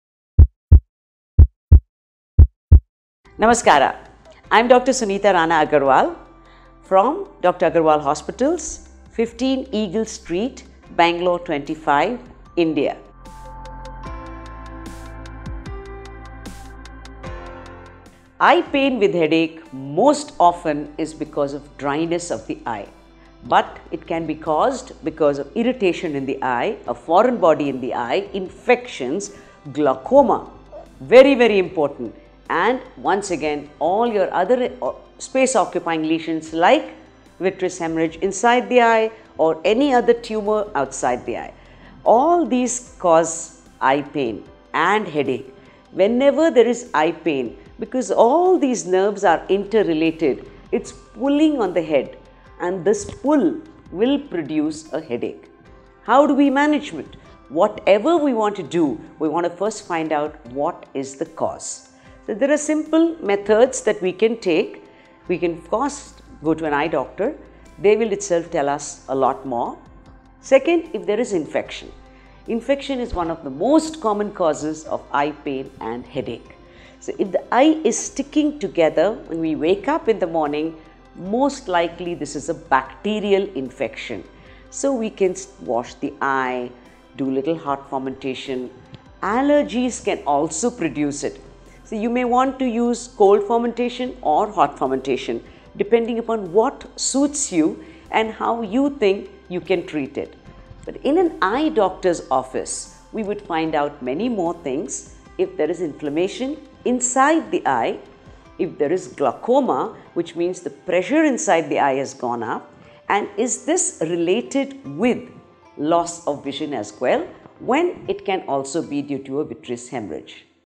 What causes eye pain with headache? - Dr. Sunita Rana Agarwal
Eye pain with headache is most probably because of dryness of the eye, but it can be a cause by irritation in the eye, foreign body in the eye, infections, glaucoma. Any space infection in the eye like the vitreous hemorrhage or any other tumor outside the eye. All these cause headache and eye pain because all these nerves are interrelated. It is pulling on the head and this pull will produce the headache. So whatever we want to do, we have to find the cause. There are simple methods that you can take. First is go to an eye doctor, they will tell you a lot more. Second is the infection. Infection is one of the most common causes of eye pain with headache. If the eye is sticking together when we wake up in the morning, then most probably it is a bacterial infection. So we have to wash the eye. Do little hot fermentation. Allergies can also produce this. So you may want to do a little cold fermentation and hot fermentation depending on what suits you and how you think you can treat it.  In an eye doctor’s office, you can find out more other things like if there is an inflammation inside the eye. If there is glaucoma, that means if the pressure in the eye has gone up and is this related with loss of vision as well, when it can be due to a vitreous hemorrhage.